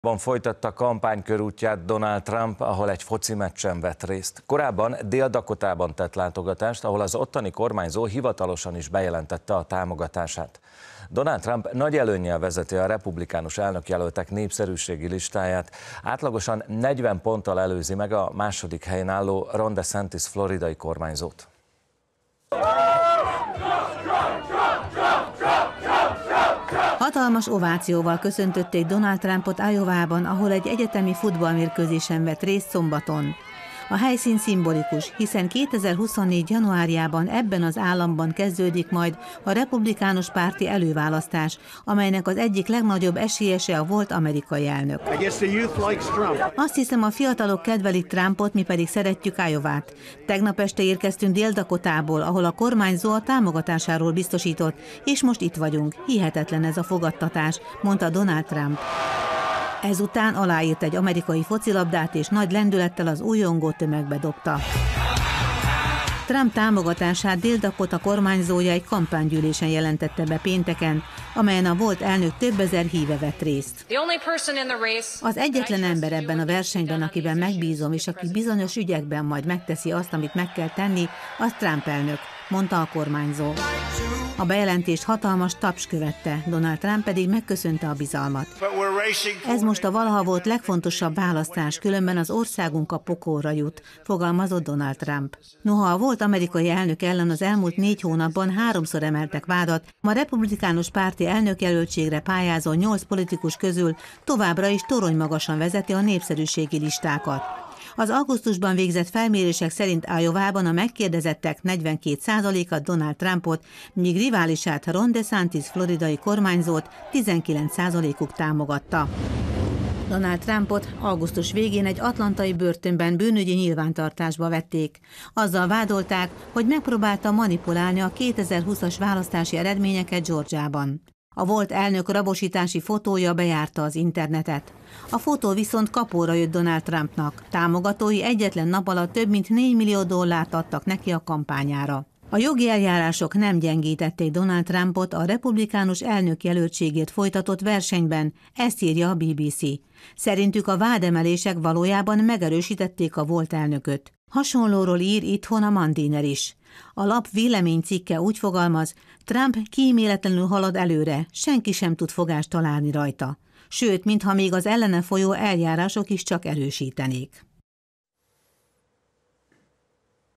Iowában folytatta kampánykörútját Donald Trump
Iowában folytatta kampánykörútját Donald Trump, ahol egy focimeccsen vett részt. Korábban Dél-Dakotában tett látogatást, ahol az ottani kormányzó hivatalosan is bejelentette a támogatását. Donald Trump nagy előnnyel vezeti a republikánus elnökjelöltek népszerűségi listáját, átlagosan 40 ponttal előzi meg a második helyen álló Ron DeSantis floridai kormányzót.